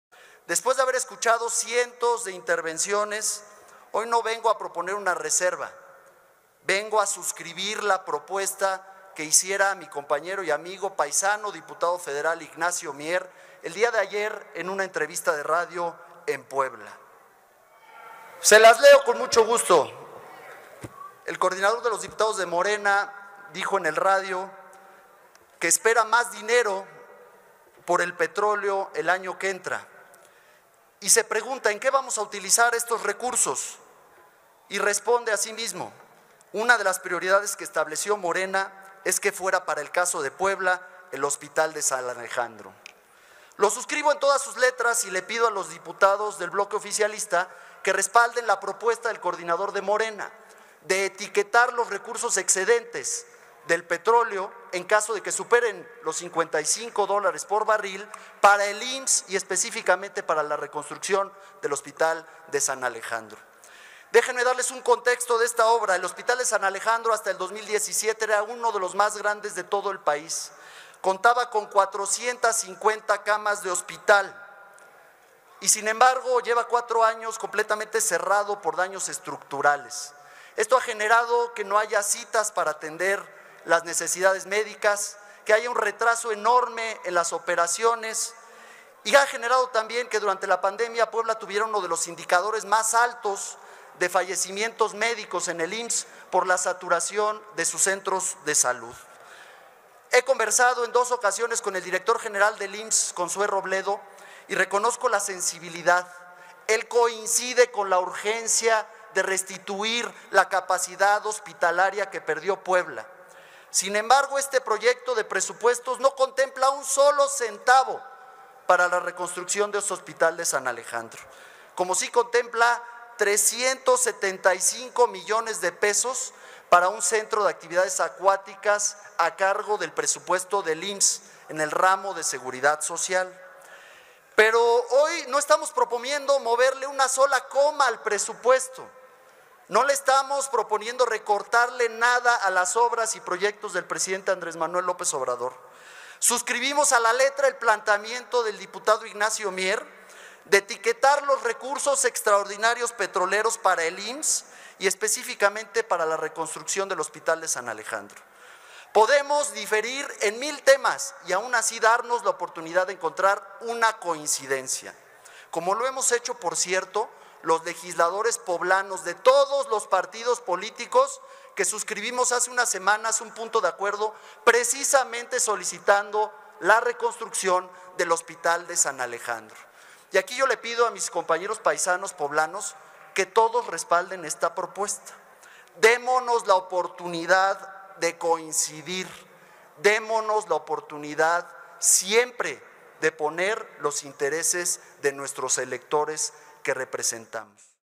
Denuncia Mario Riestra que bancada de Morena desechó la propuesta para reconstruir San Alejandro
Denuncia Mario Riestra que la bancada de Morena desechó la propuesta de etiquetar los excedentes pretroleros para reconstruir San Alejandro